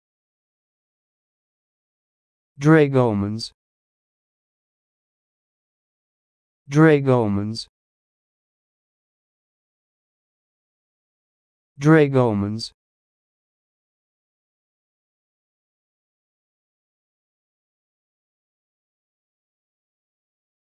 Learn how to say this word: "Dragomans"
This is the definition of "Dragomans":
pl.   of Dragoman